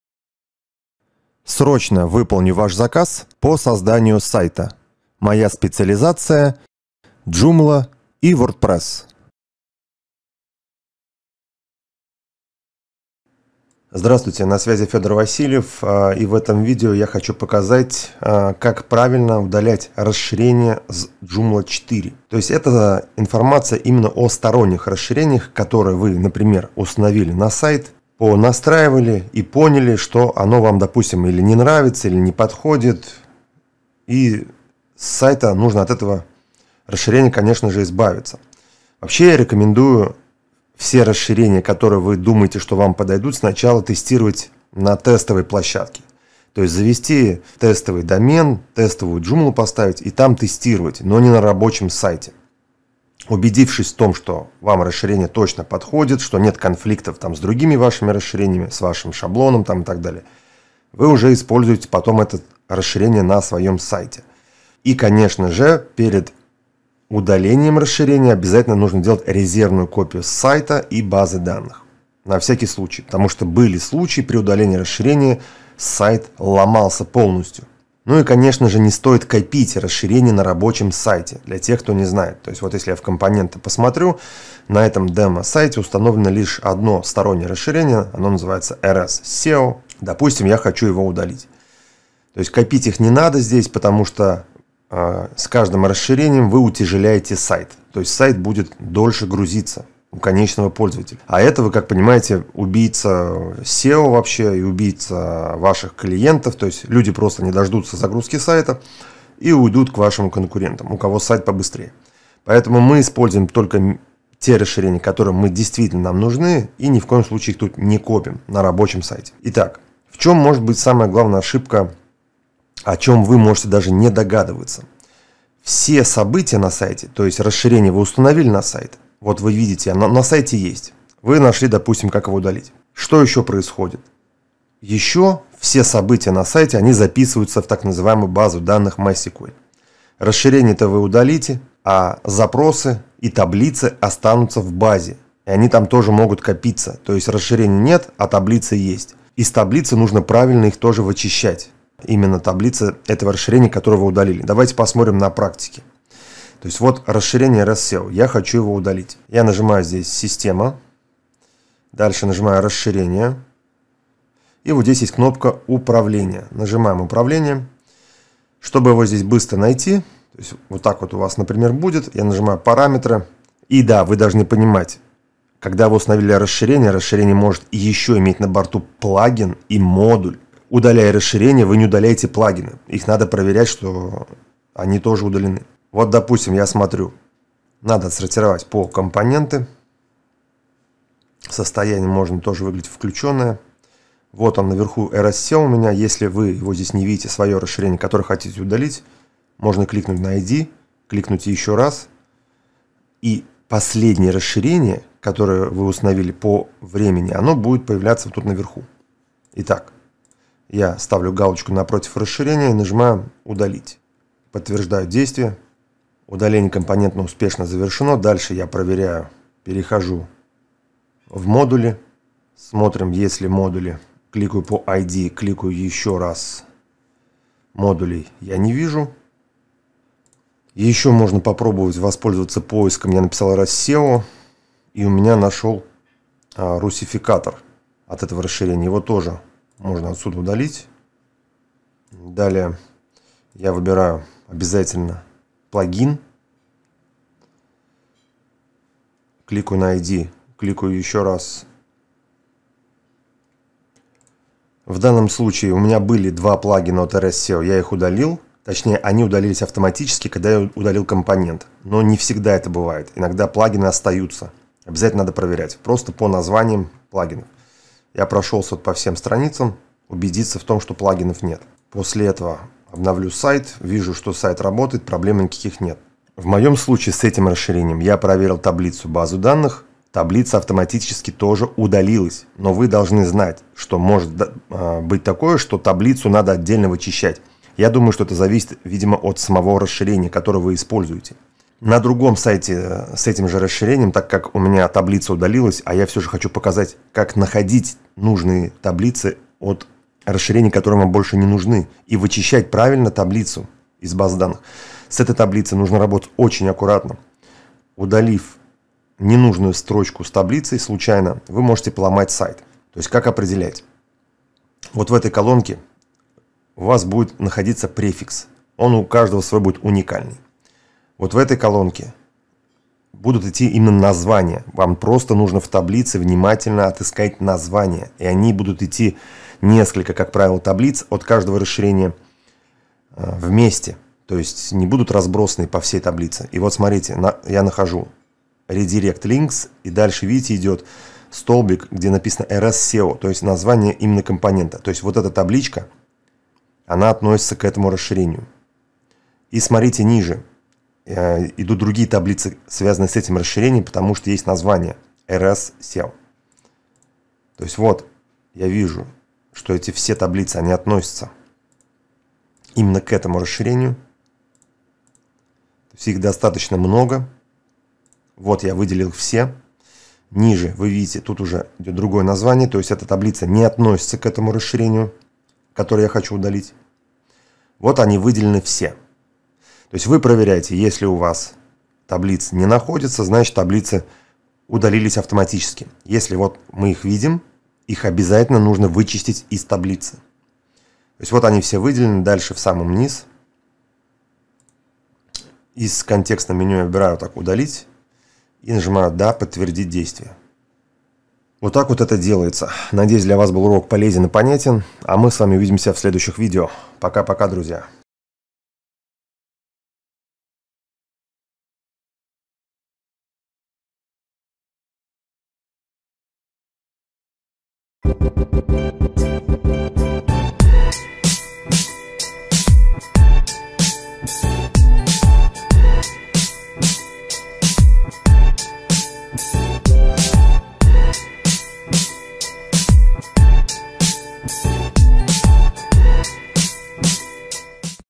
Как правильно удалять расширения в Joomla 4
При возникновении ситуации, когда требуется удалить то или иное стороннее расширение Joomla 4, нужно понимать,
что удаляя например компонент, не всегда удаляется его плагины, модули а так же его таблицы в базе данных MySQL.